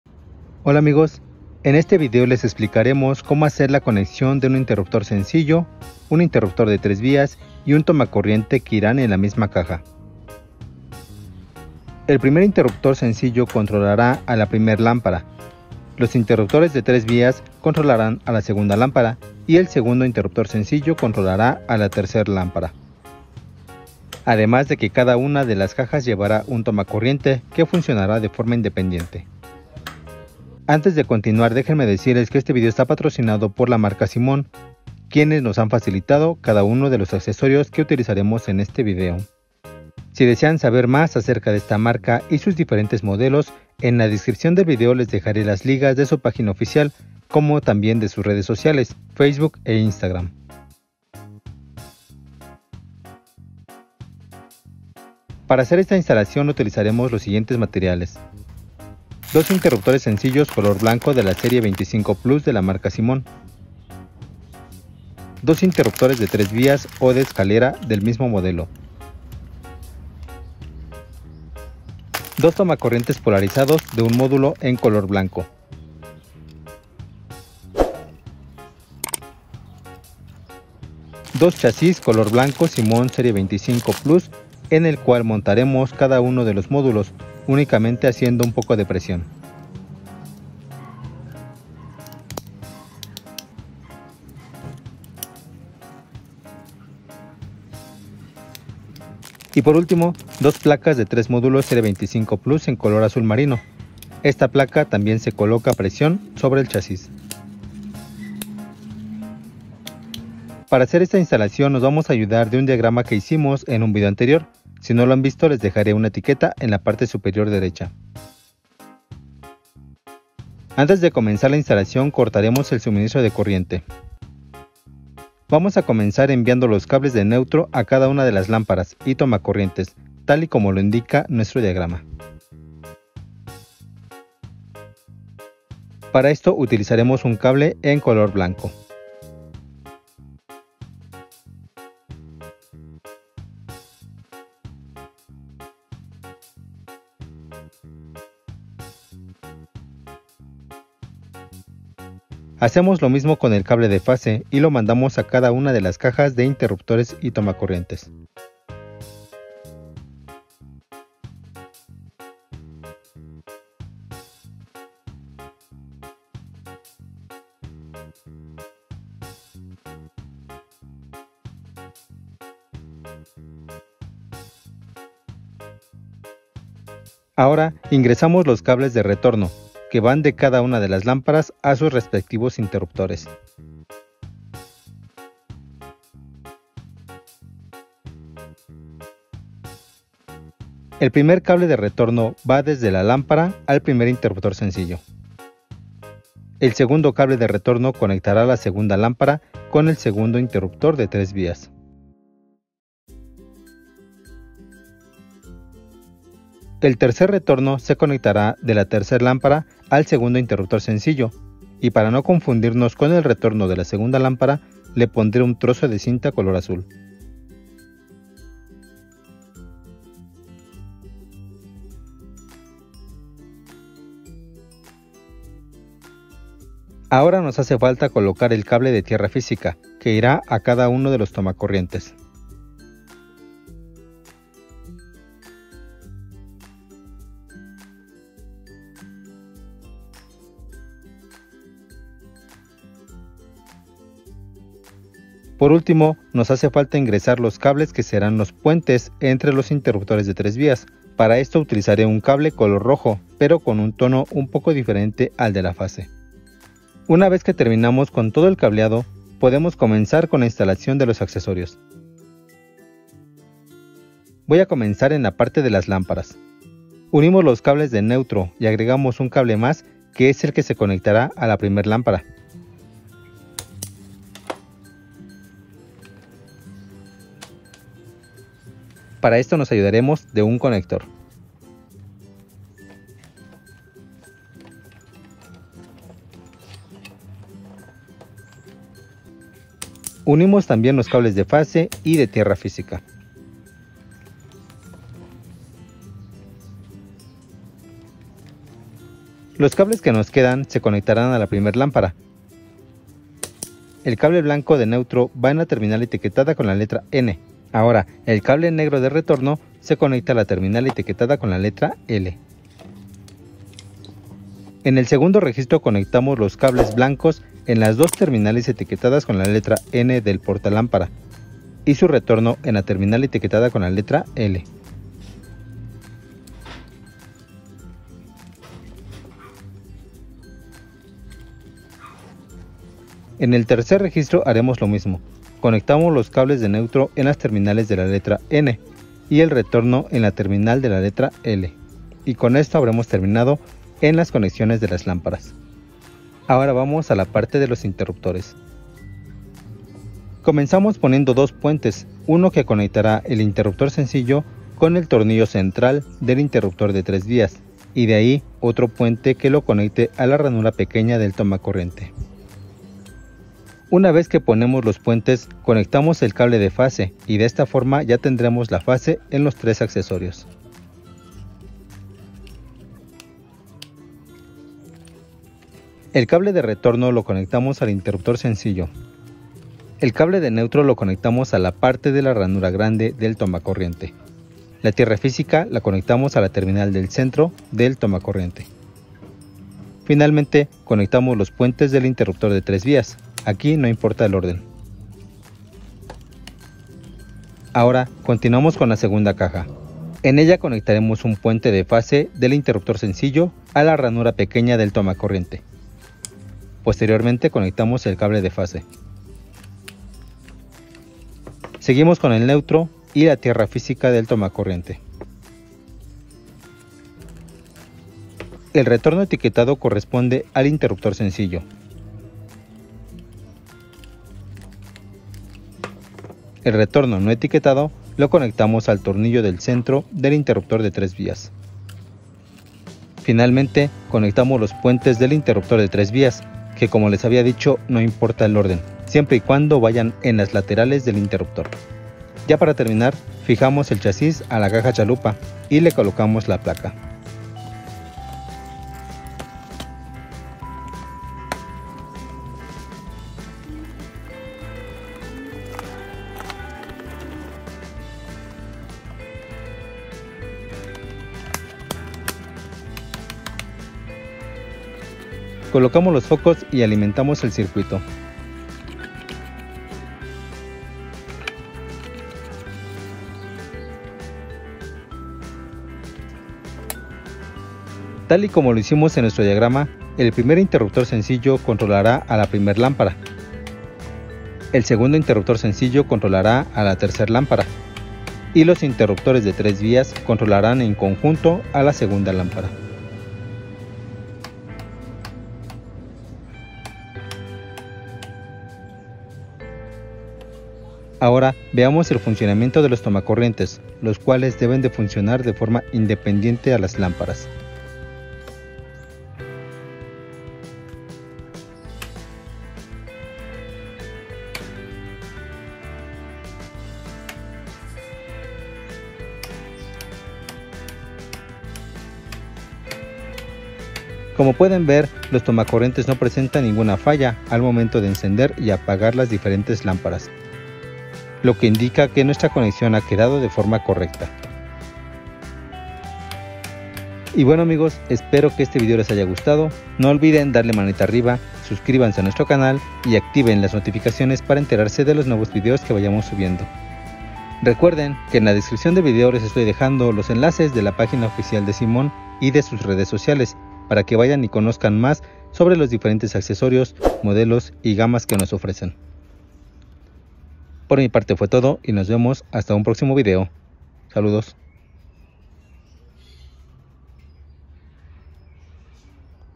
Cómo conectar un interruptor sencillo, interruptor de 3 vías y tomacorriente en la misma caja
No olvides visitar las páginas oficiales de Simón para conocer su amplia gama de accesorios.